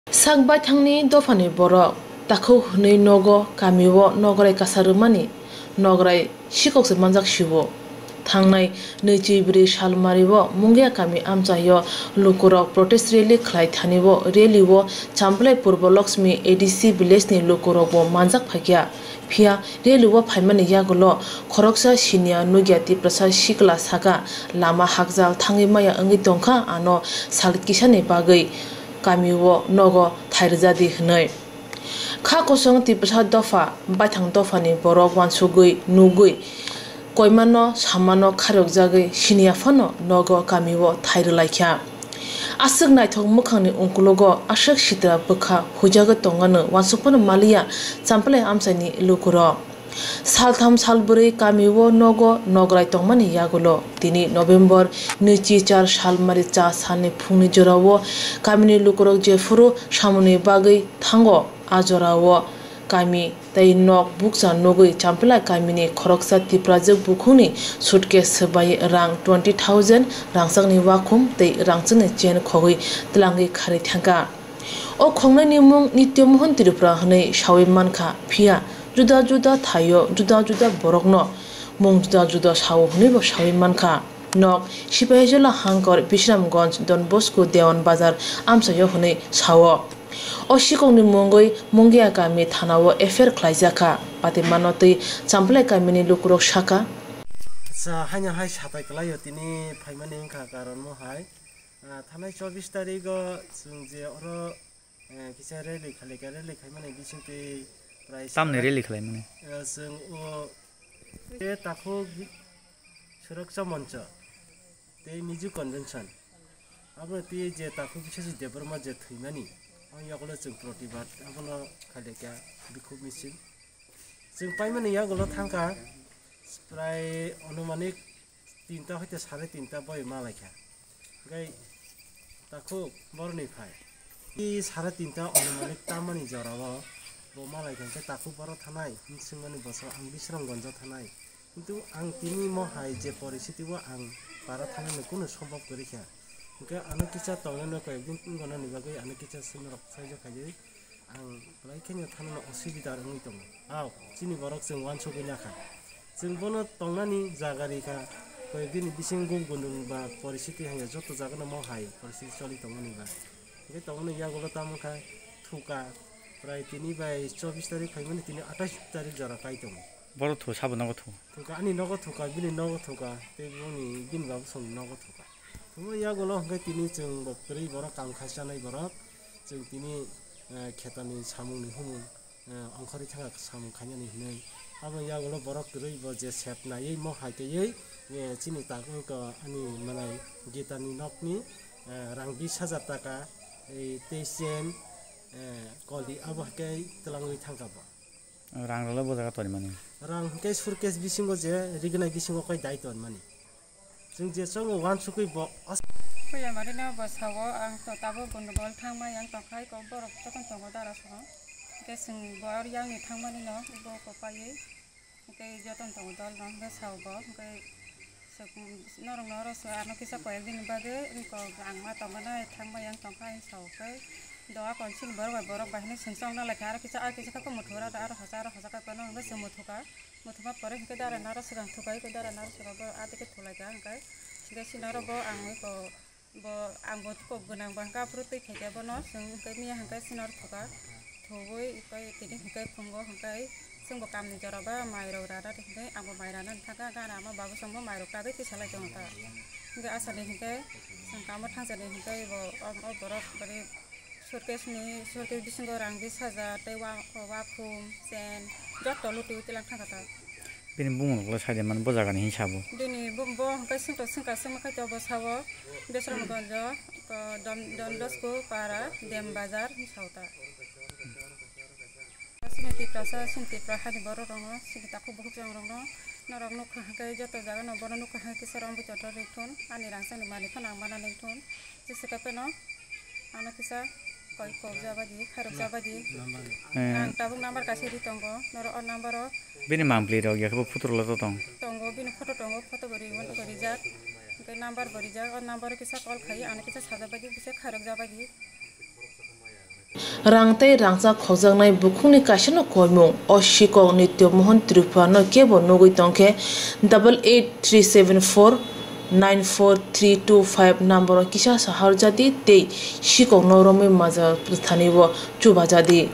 Sikhoknose Nograi kasa rwsio mungia kami champlai kamini lukurog.Khogwi twlangkha Rang tei Rangchak,
champlai kamini khoroksa Twiprajwk bukhukni Sutkesh swbaiwi rang 20.000, Wakhum, tei rangchakni Chan  khogwi twlangwi kharwi thangkha. khoknani mung Nitya Mahon Tripura. phiya juda juda borok juda mung sao hwnwibo sawi mankha, nok Sepahijala hangkor Bisrangonj Donbosco Dewan basaro hwnwi sawi mankha. O sikhokni mungwi Mungia kami thanao FIR khwlaijakha..